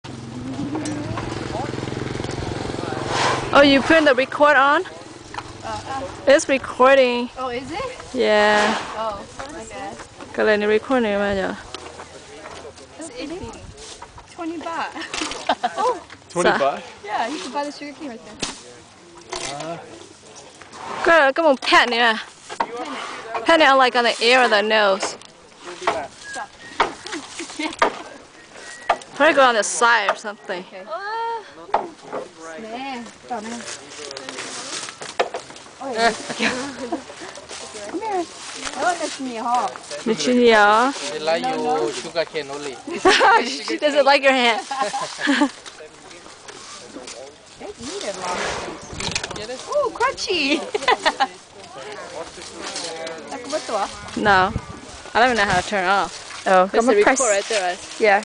Elephant feeding in CHiang Mai, Thailand
This is me feeding the baby elephant up in the hills of Chiang Mai in Thailand.